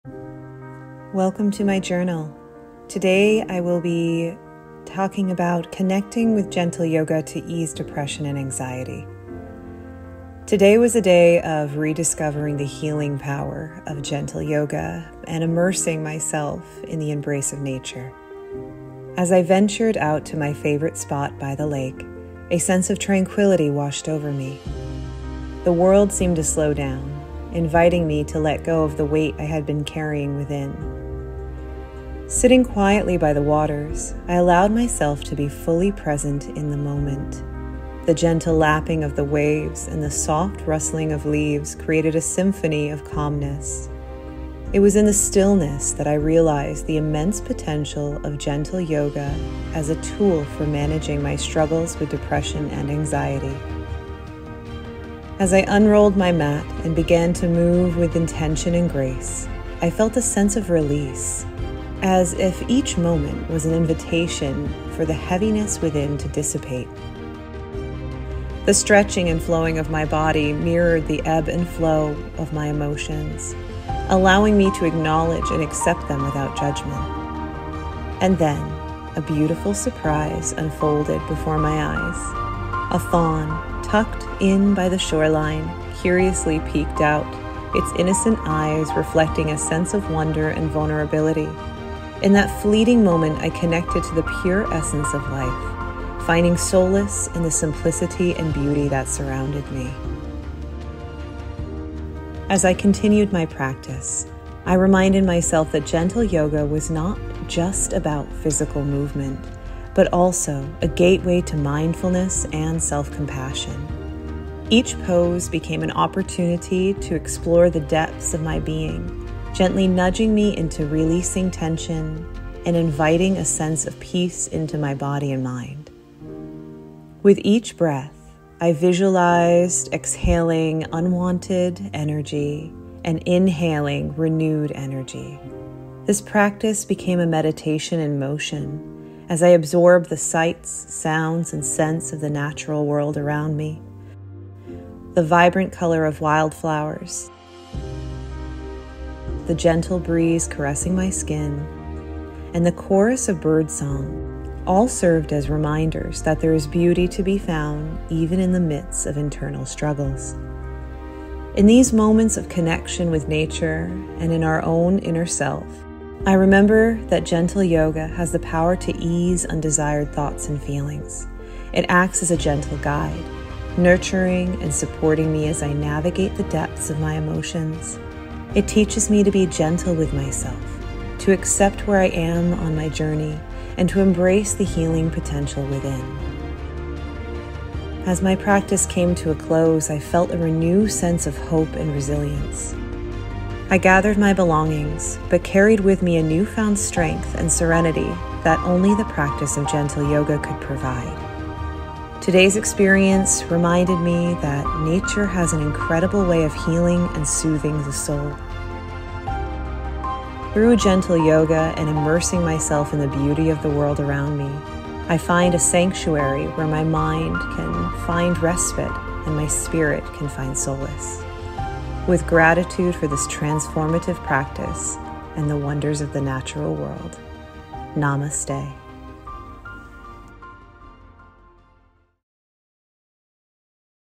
Discovering Serenity: Gentle Yoga and the Healing Power of Nature
Welcome to my channel, where I invite you to join me on a journey of self-discovery and inner healing through the practice of gentle yoga and a deep connection with nature. In this heartfelt journal entry, I share my personal experience of rediscovering the profound healing power of gentle yoga in the embrace of the natural world.

As I ventured to my favorite spot by the serene lake, a profound sense of tranquility enveloped me, urging me to release the burdens I had carried within. In the stillness, I realized the immense potential of gentle yoga as a transformative tool for managing depression and anxiety. With each intentional movement and mindful breath, I felt a beautiful release, as if the heaviness within was dissipating.

But it was not just the physical practice of gentle yoga that moved me; it was the gateway it opened to mindfulness and self-compassion. Each pose became an opportunity to explore the depths of my being, gently guiding me to release tension and invite peace into my body and mind. The rhythmic flow of my movements mirrored the ebb and flow of my emotions, allowing me to acknowledge and accept them without judgment.

And then, nature bestowed upon me a precious gift—a fawn cautiously peering out at me. In that fleeting moment, I connected with the pure essence of life and found solace in the simplicity and beauty that surrounded me. It was a gentle reminder that even amidst our struggles, there is boundless beauty to be found.

Through these profound connections with nature and my own inner self, I have come to understand that gentle yoga is a gentle guide, nurturing and supporting us as we navigate the depths of our emotions. It teaches us to be gentle with ourselves, accepting where we are on our individual journeys and embracing the healing potential within.

Today's experience has reminded me of nature's remarkable ability to heal and soothe the soul. It has reaffirmed the transformative power of gentle yoga and its ability to provide a sanctuary where the mind can find respite and the spirit can find solace.

I am grateful to share this transformative practice and the wonders of the natural world with you. Join me on this journey of self-discovery, healing, and connection. Together, let us find solace and embrace the beauty that surrounds us.